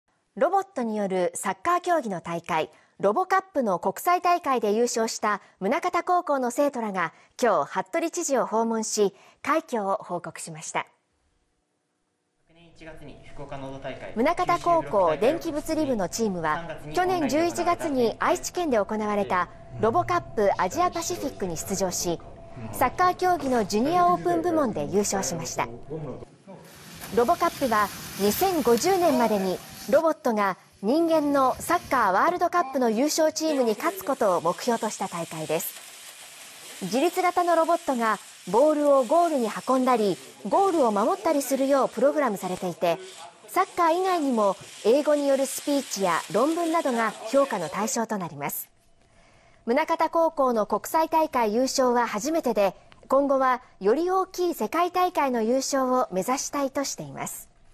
「ロボカップ」優勝の宗像高校が知事表敬
ロボットによるサッカー競技の大会、「ロボカップ」の国際大会で優勝した宗像高校の生徒らが、１８日、服部知事を訪問し、快挙を報告しました。

宗像高校電気物理部のチームは、去年１１月に愛知県で行われた「ロボカップアジアパシフィック」に出場し、サッカー競技のジュニアオープン部門で優勝しました。

「ロボカップ」は２０５０年までに、ロボットが人間のサッカーワールドカップの優勝チームに勝つことを目標とした大会です。

自律型のロボットがボールをゴールに運んだり、ゴールを守ったりするようプログラムされていて、サッカー以外にも、英語によるスピーチや、論文などが評価の対象となります。

宗像高校の国際大会優勝は初めてで、今後は、より大きい世界大会の優勝を目指したいとしています。